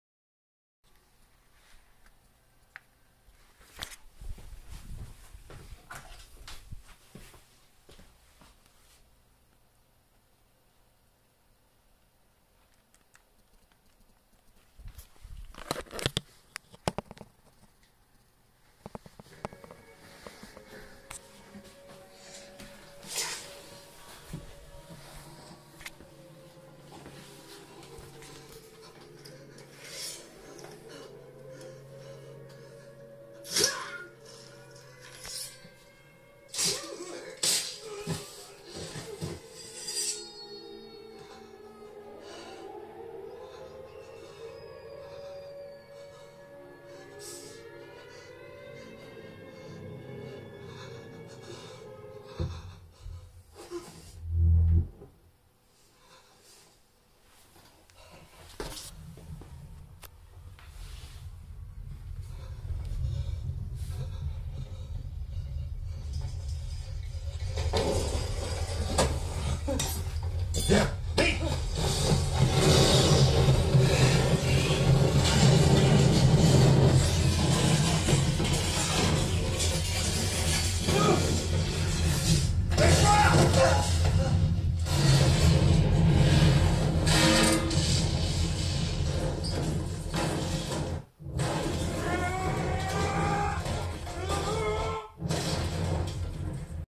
Cerwin vega xls 6c only
Petite video rapide de cette enceinte en configuration de film